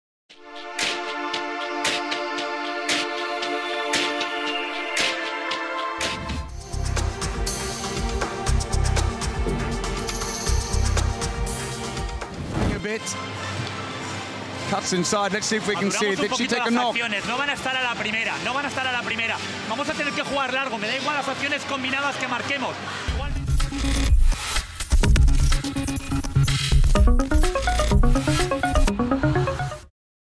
Anna Vyakhireva: Playing one-on-one after a counter-attack | Video analysis | IHF Education Centre
Video analysis: 2019 IHF Women's World Championship
Player profile of Anna Vyakhireva, right back (RUS)
Playing one-on-one after a counter-attack — Well-controlled speed, feint and subsequent shot on goal